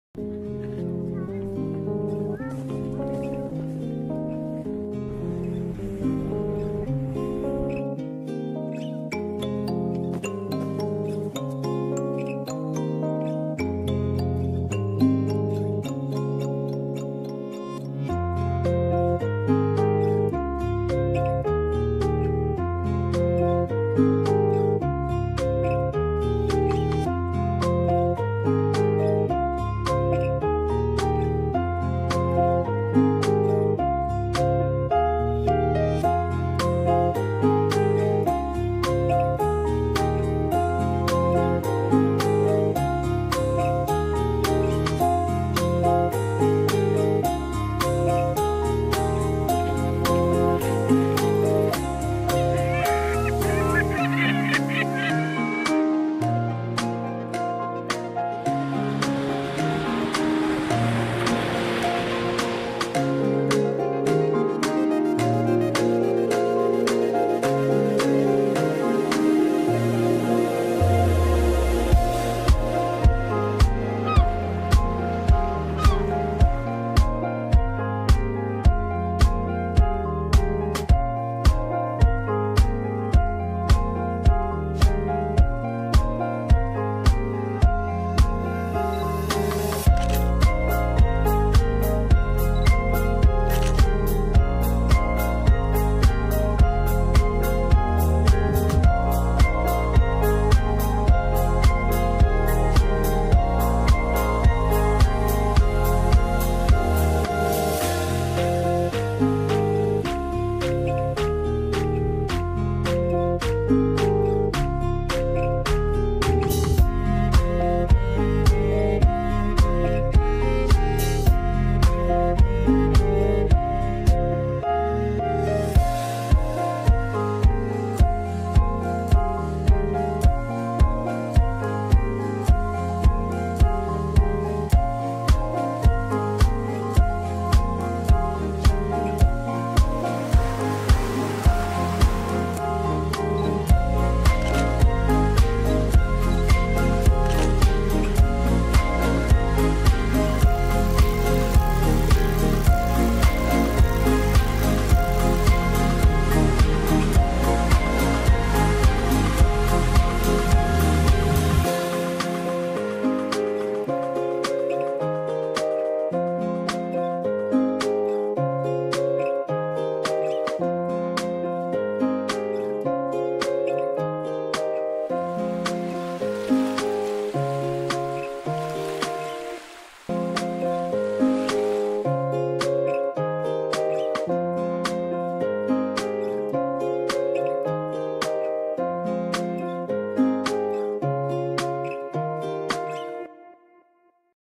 L'île d'Yeu à Vélo 🚲 [Microw / Dji osmo pocket]
Visite de l'île d'Yeu à vélo lors d'un weekend ensoleillé. En espérant que ça vous plaise.

Filmed with dji osmo pocket in 4k